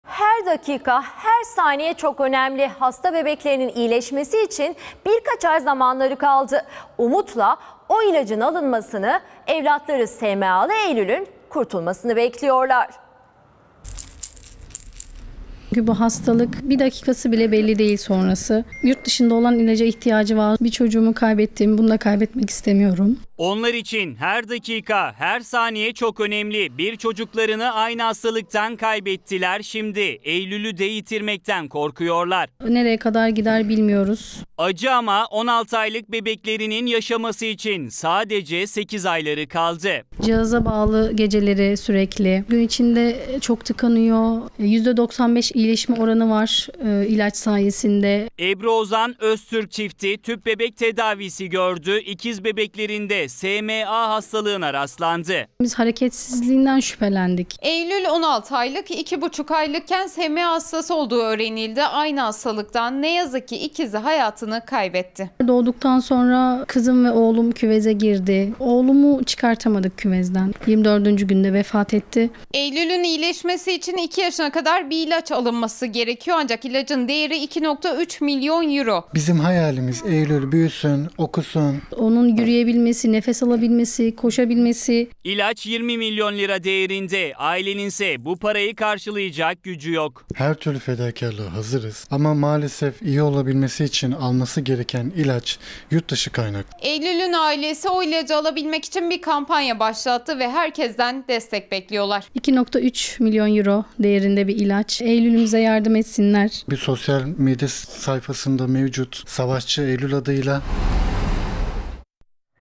Eylül'ü yaşatabilmek için 8 ay kaldı
HER DAKİKA HER SANİYE ÇOK ÖNEMLİ... HASTA BEBEKLERİNİN İYİLEŞMESİ İÇİN BİRKAÇ AY ZAMANLARI KALDI... UMUTLA O İLACIN ALINMASINI EVLATLARI SMA'LI EYLÜL 'ÜN KURTULMASINI BEKLİYORLAR.